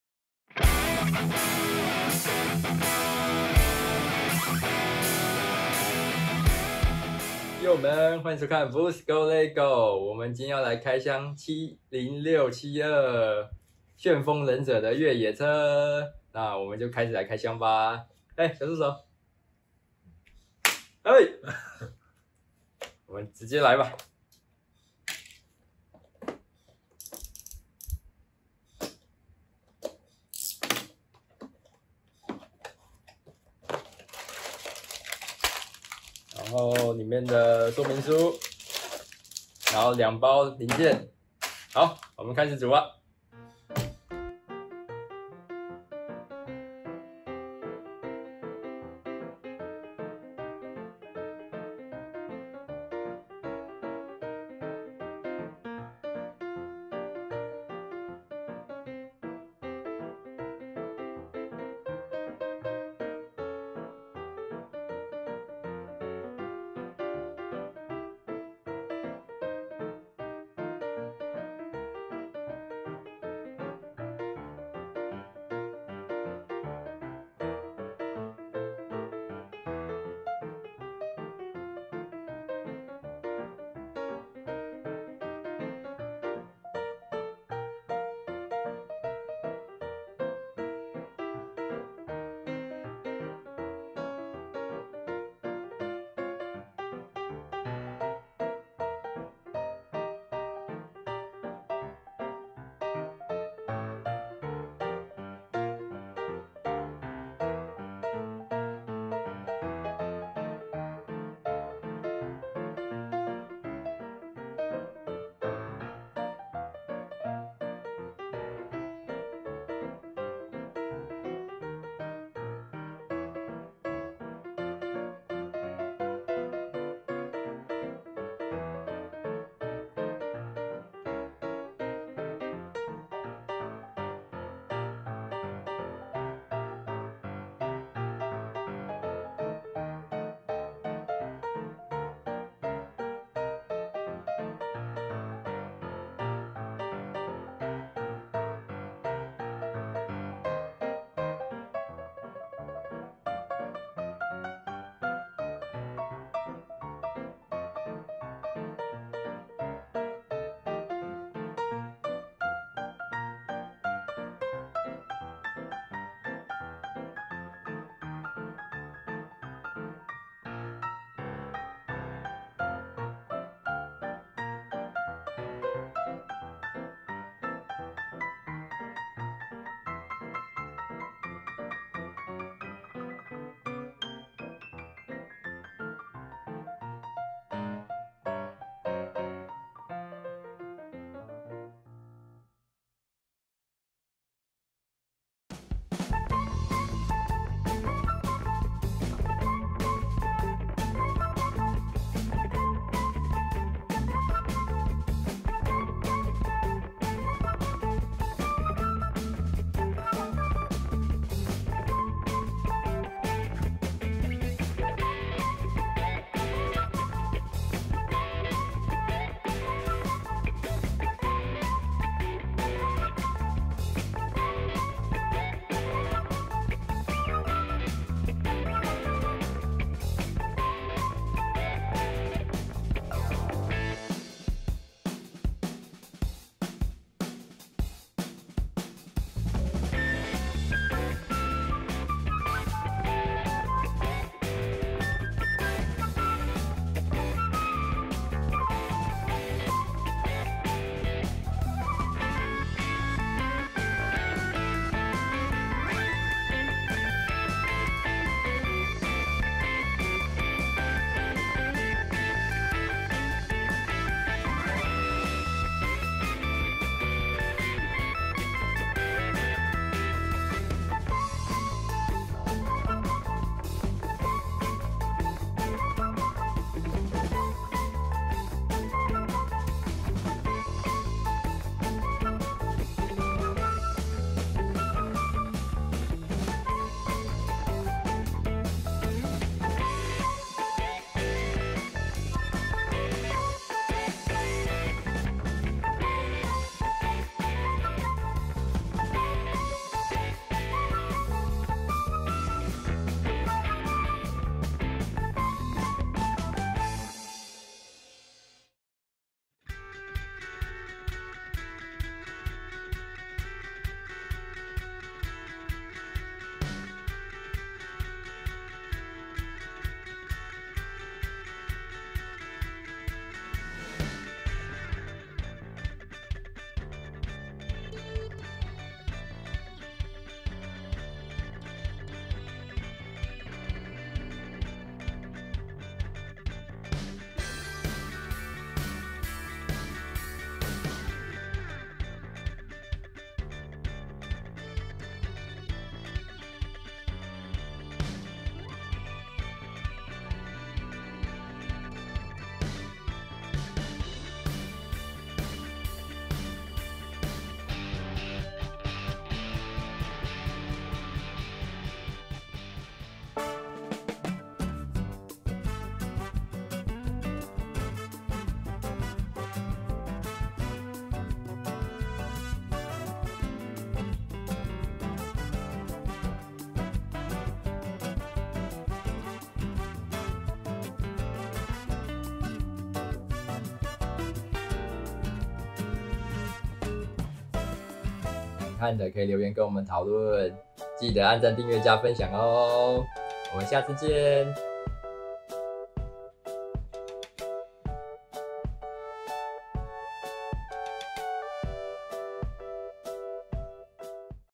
【BLUCE GO LEGO】 EP7 Comes the Ninjago~Unboxing LEGO 70672 Cole's Dirk Bike 旋風忍者來襲~開箱樂高70672(Eng cc)
歡迎來到 BLUCE  GO LEGO
這集是要開箱樂高70672
旋風忍者Cole的越野車
組裝並展示成品

Welcome to BLUCE GO LEGO.
EP7 will unbox the LEGO 70672.
Ninjago Cole' Dirk Bike
Build & Demo the goods

別忘記開鈴鐺~~~開起來~~~

Time Code
0:05 開箱 (Unboxing)
0:44 組裝 (Build)
6:03 展示 (Show the goods)
6:25 結語 (Ending)

相關資訊參考網路相關資訊
若有侵犯到版權煩請告知